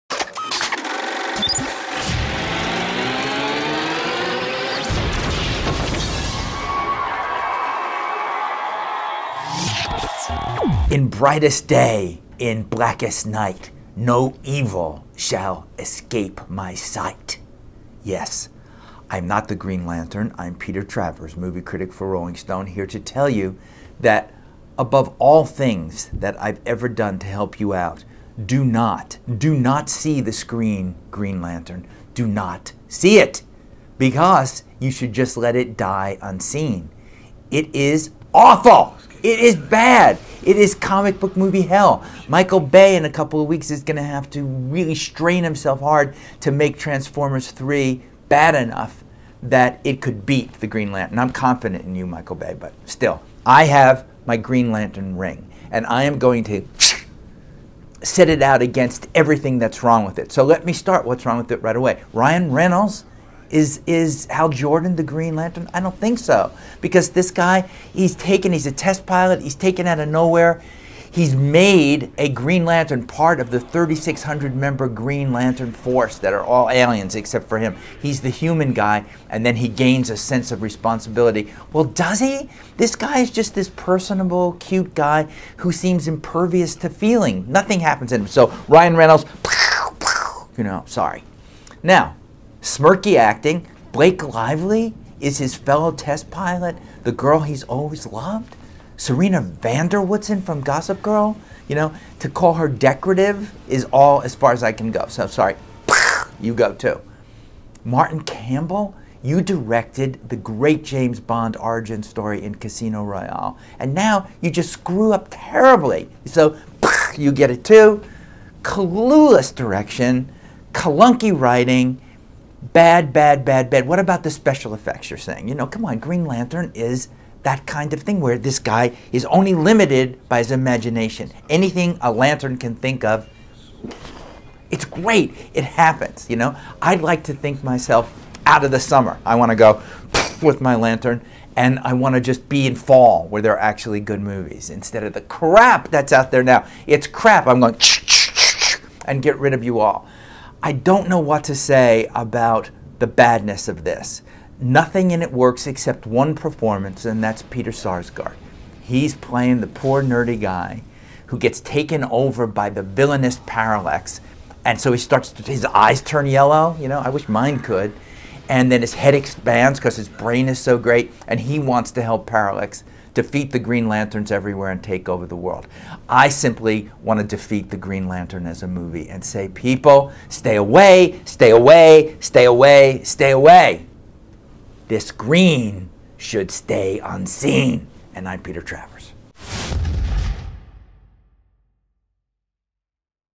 'Green Lantern' Welcome to 'comic book movie hell'
Peter Travers is urging everyone to skip Green Lantern, the movie based on DC Comics' sci-fi super hero starring Ryan Reynolds. &quot;It is awful, it is bad, it is comic book movie hell,&quot; says Travers. &quot;Michael Bay in a couple weeks is going to have to strain himself hard to make Transformers 3 bad enough that it can beat the Green Lantern.&quot; Though most everything about the movie is a mess, Travers does approve of Peter Sarsgaard, who turns in the picture's only good performance as the mutated villain Hector Hammond.